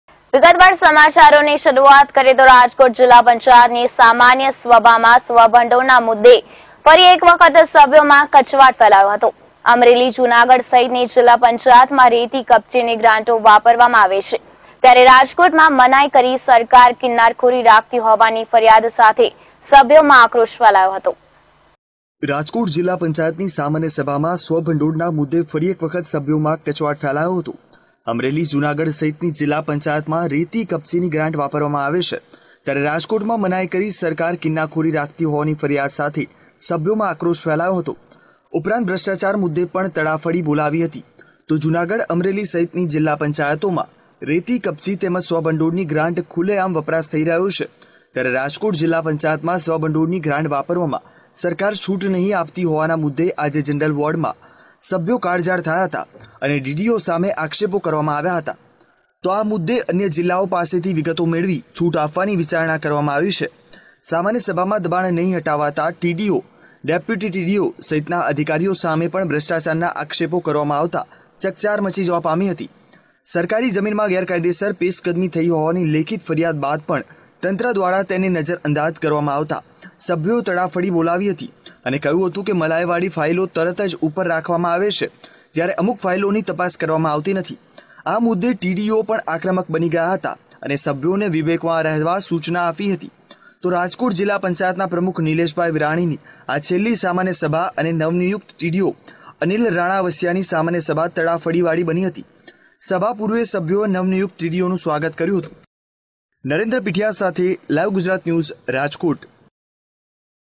RAJKOT MA JILLA PANCHAYAT NI YOJAI SAMANYA SABHA
રાજકોટ જીલ્લા પંચાયતની સામાન્ય સભામાં સ્વભંડોળના મુદ્દે ફરી એક વખત સભ્યોમાં કચવાટ ફેલાયો હતો. અમરેલી, જૂનાગઢ સહિતની જીલ્લા પંચાયતમાં રેતી, કપચીની ગ્રાંટ વાપરવામાં આવે છે ત્યારે રાજકોટમાં મનાઇ કરી સરકાર કિન્નાખોરી રાખતી હોવાની ફરીયાદ સાથે સભ્યોમાં આક્રોશ ફેલાયો હતો. ઉપરાંત ભ્રષ્ટાચાર મુદ્દે પણ તડાફડી બોલાવી હતી.જૂનાગઢ, અમરેલી સહિતની જીલ્લા પંચાયતોમાં રેતી, કપચી તેમજ સ્વભંડોળની ગ્રાંટ ખુલ્લેઆમ વપરાશ થઇ રહ્યો છે. જ્યારે રાજકોટ જીલ્લા પંચાયતમાં સ્વભંડોળની ગ્રાંટ વાપરવામાં સરકાર છુટ નહીં આપતી હોવાના મુદ્દે આજે જનરલ બોર્ડમાં સભ્યો કાળઝાળ થયા હતા અને ડીડીઓ સામે આક્ષેપો કરવામાં આવ્યા હતા. આ મુદ્દે અન્ય જીલ્લા પાસેથી વિગતો મેળવી છુટ આપવાની વિચારણા કરવામાં આવી છે.સામાન્ય સભામાં દબાણ નહીં હટાવતા ટીડીઓ, ડે.ડીડીઓ સહિતના અધિકારીઓ સામે પણ ભ્રષ્ટાચારના આક્ષેપો કરવામાં આવતા ચકચાર મચી જવા પામી હતી. સરકારી જમીનમાં ગેરકાયદેસર પેશકદમી થઇ હોવાની લેખિત ફરીયાદ બાદ પણ તંત્ર દ્વારા તેને નજર અંદાજ કરવામાં આવતા સભ્યોએ તડાફડી બોલાવી હતી અને કહ્યું હતું કે મલાઇવાળી ફાઇલો તરત જ ઉપર આવે છે જ્યારે અમુક ફાઇલોની તપાસ કરવામાં આવતી નથી. આ મુદ્દે ડીડીઓ પણ આક્રમક બની ગયા હતા અને સભ્યોને વિવેકમાં રહેવા સુચના આપવામાં આવી હતી.રાજકોટ જીલ્લા પંચાયતના પ્રમુખ નિલેશભાઇ વિરાણીની આ છેલ્લી સામાન્ય સભા અને નવનિયુકત ડીડીઓ અનિલ રાણાવસીયાની સામાન્ય સભા તડાફડીવાળી બની હતી. સભા પૂર્વે સભ્યોએ નવનિયુકત ડીડીઓનું સ્વાગત કર્યુ હતું.